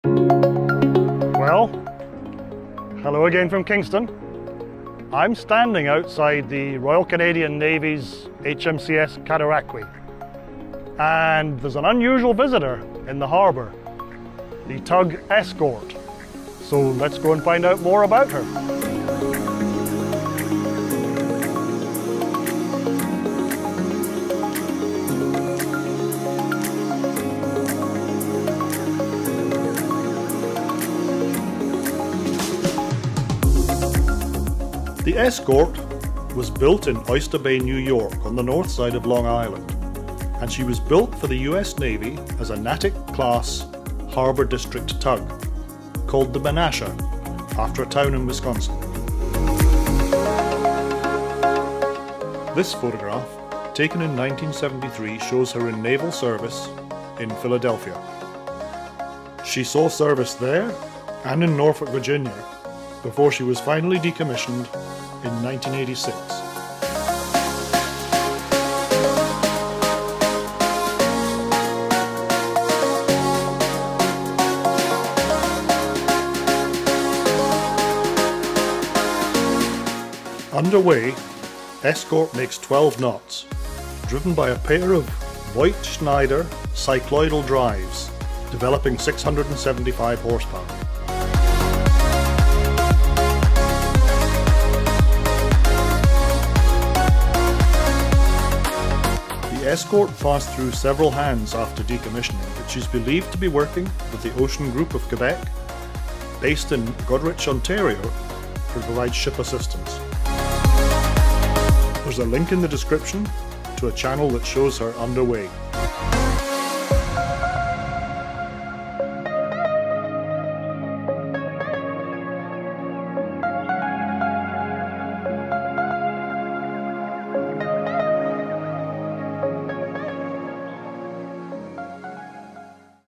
Whatever happened to the USS Menasha ? 4K
This short video introduces viewers to the Tractor Tug 'Escorte' - a Montreal-registered vessel providing ship assistance on Lake Huron out of Goderich, Ontario.  She was launched in November 1964 to serve the US Navy, which she did faithfully until 1986. Since then, she has been in civilian service.

Learn more about her unusual drive arrangements and see it in action in this animated video